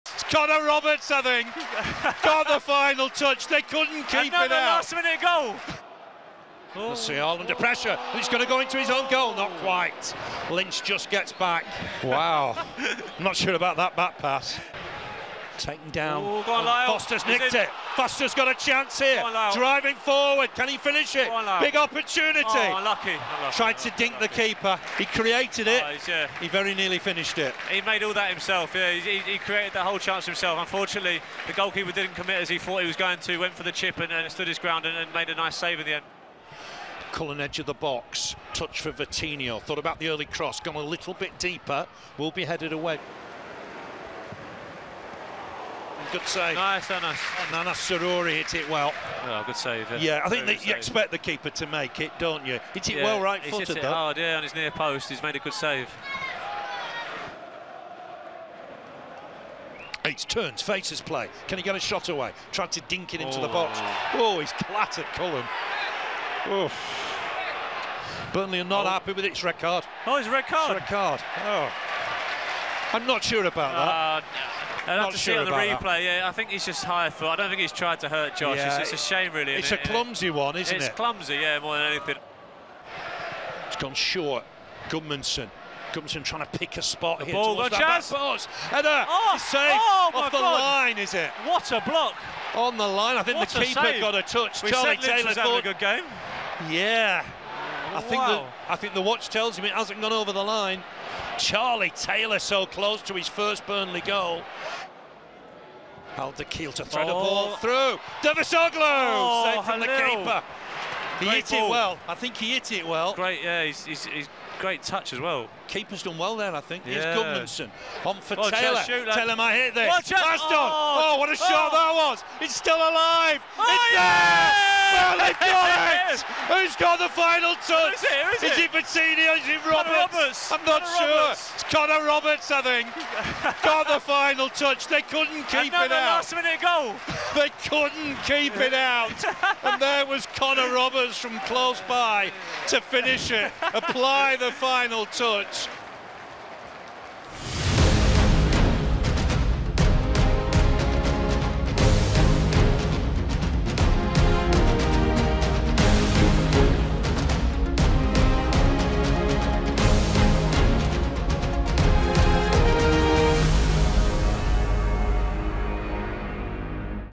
LAST MINUTE DRAMA! | FA Cup Highlights | Burnley v Fleetwood Town
Roberts late goal seals quarter-final spot for the Clarets!

Find even more videos, as well as live match streams and commentary for Burnley's 2022/23 Championship season on Clarets+